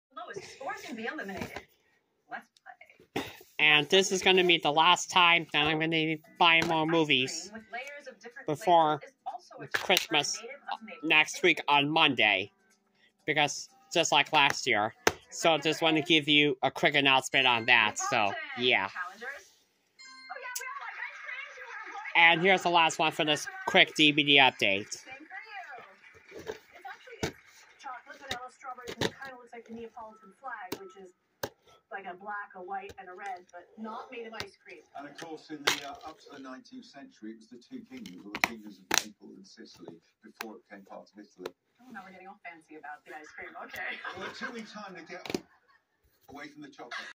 quick DVD update/announcement video for December 19th, 2023
Note: those are the only 2 DVDs I got at work today, also this is the last time I am buying more movies before Christmas on Monday.